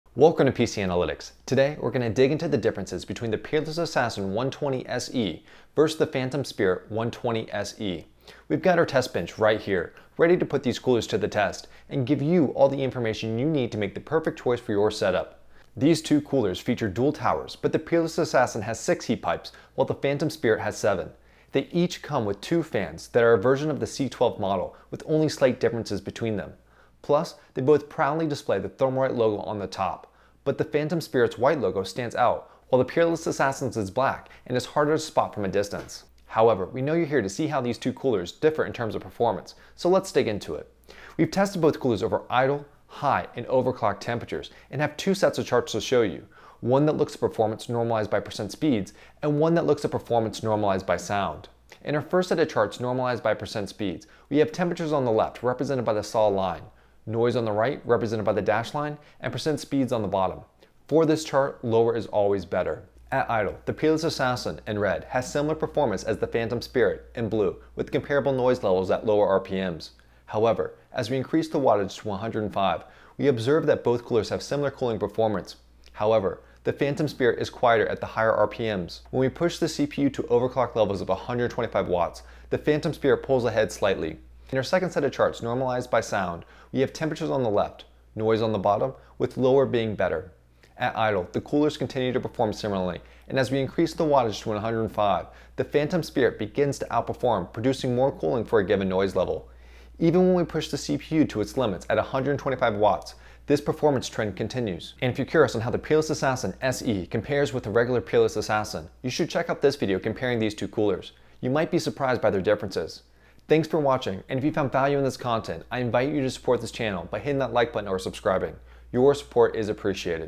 Review: Peerless Assassin 120 SE vs Phantom Spirit 120 SE
This videos compares the the Thermalright Peerless Assassin 120 SE vs the Phantom Spirit 120 on both qualitative factors and performance.

Peerless Assassin 120 SE
Phantom Spirit 120 SE